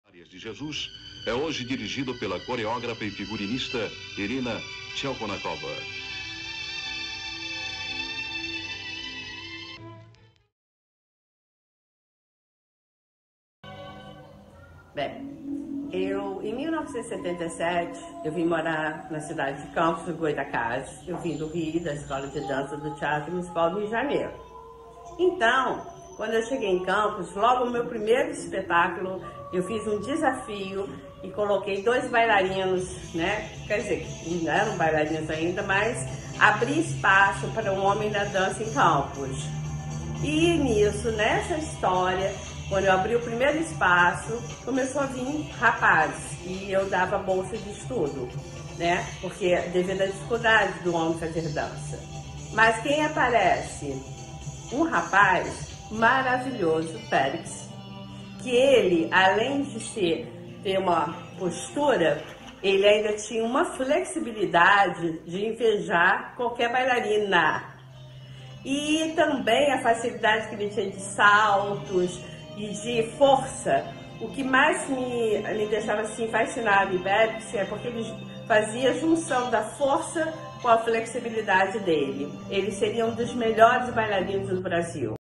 Professora Marcia Cristina Lisboa fala sobre Pericles Emmanuel celebracao 60 anos .
A renomada Professora de varias modalidades de danca radicada em Campos dos Goytacazes a decadas fala sobre o artista Pericles Emmanuel The Brazilian Jazz Singer celebracao dos 60 anos do artista.